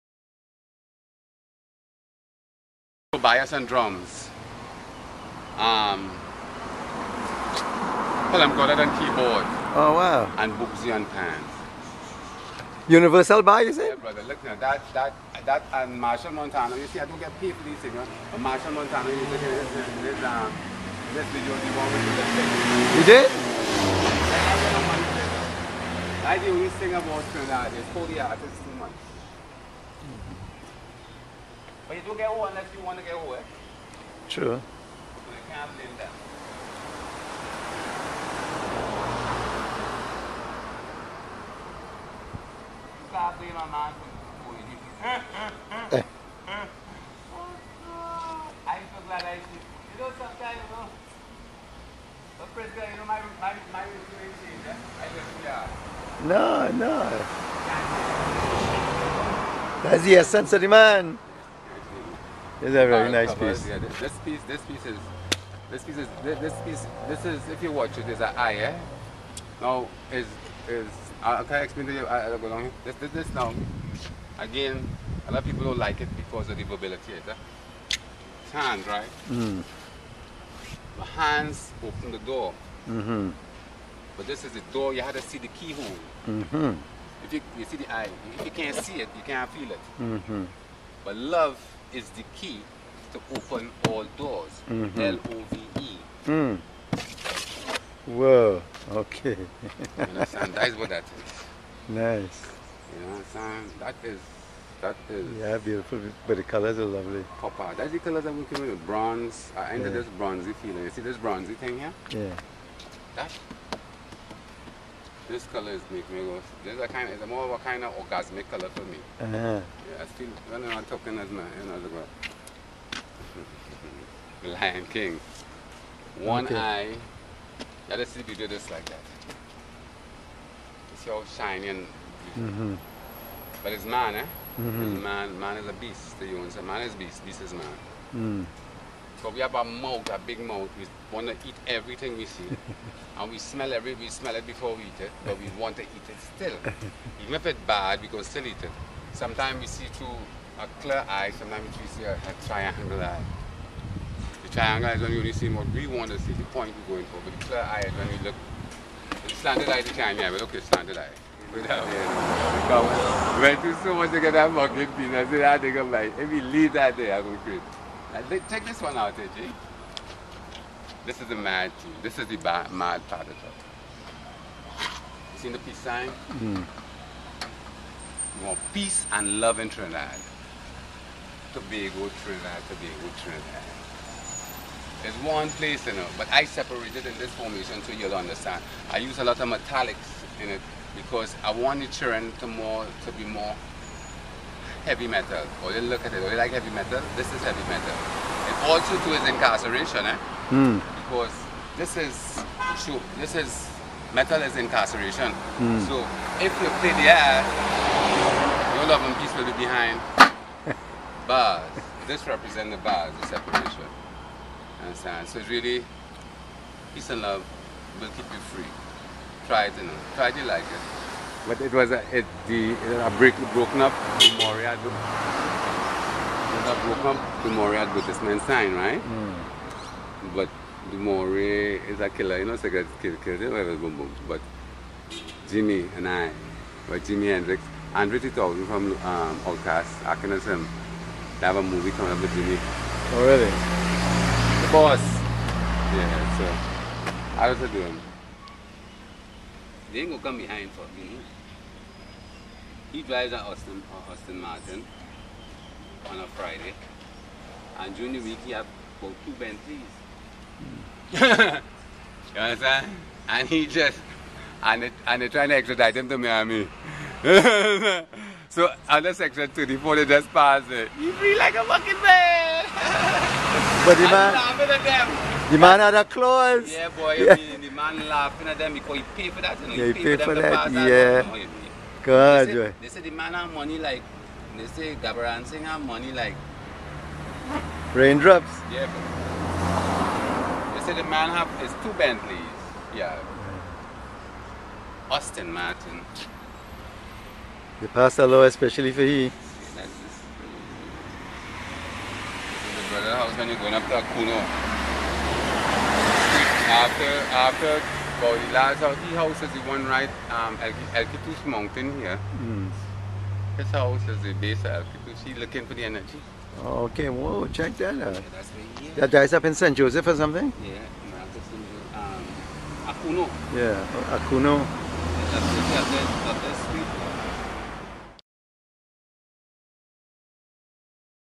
Trevor St. George: Love Is The Door That Opens All Doors.mp4
Petra Street, in Port Of Spain, Trinidad is the center of his universe. Night and day Trevor St. George holds court and paints, and paints, and paints.